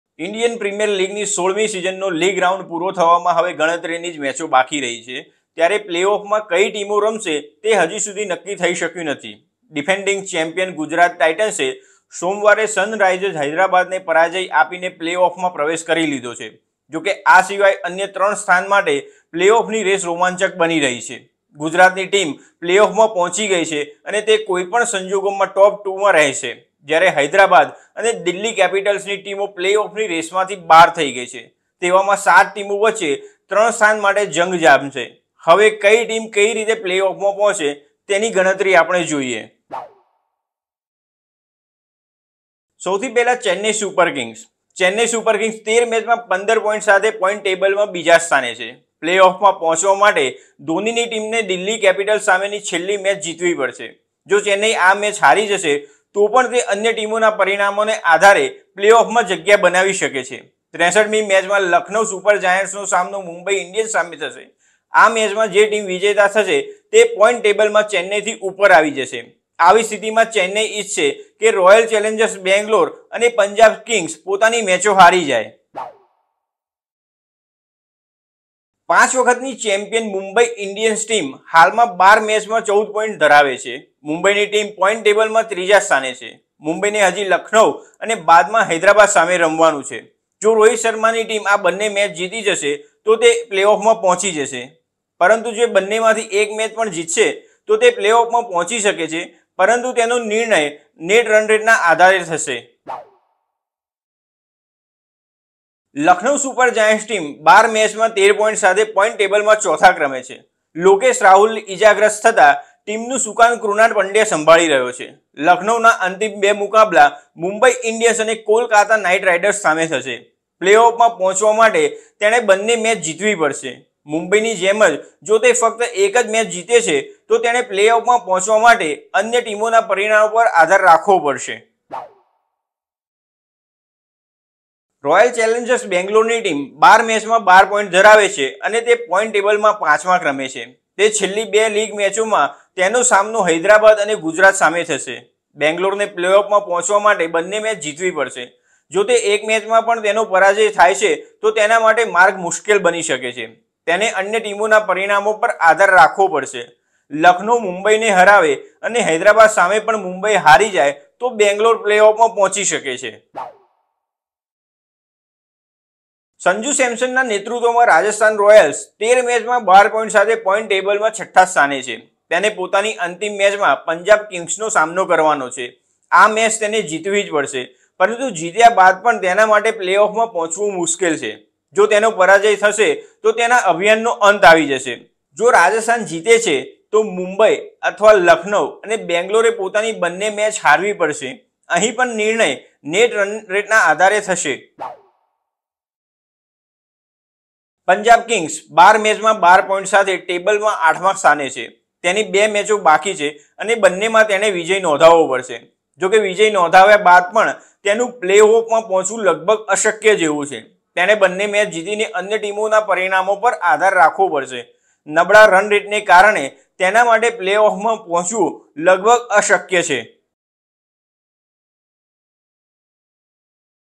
IPL 2023: સ્થાન 3, ટીમ 7 અને મેચ 8... પ્લેઓફ માટે સુપર ઓવર જેવું રોમાંચક સમીકરણ
ઈન્ડિયન પ્રીમિયર લીગ (Indian Premier League)ની 16મી સિઝનનો લીગ રાઉન્ડ પૂરો થવામાં હવે ગણતરીની જ મેચો બાકી રહી  છે. ત્યારે પ્લેઓફ IPL Playoffs)માં કઈ ટીમો રમશે તે હજી સુધી નક્કી થઈ શક્યું નથી. ડિફેન્ડિંગ ચેમ્પિયન ગુજરાત ટાઈટન્સ (Gujarat Titans) ટીમે સોમવારે સનરાઈઝર્સ હૈદરાબાદને પરાજય આપીને પ્લેઓફમાં પ્રવેશ કરી લીધો છે. જોકે, આ સિવાય અન્ય ત્રણ સ્થાન માટે પ્લેઓફની રેસ રોમાંચક બની રહી છે. ગુજરાતની ટીમ પ્લેઓફમાં પહોંચી ગઈ છે અને તે કોઈ પણ સંજોગોમાં ટોપ-2માં રહેશે. જ્યારે હૈદરાબાદ અને દિલ્હી કેપિટલ્સની ટીમો પ્લેઓફની રેસમાંથી બહાર થઈ ગઈ છે. તેવામાં સાત ટીમો વચ્ચે ત્રણ સ્થાન માટે જંગ જામશે.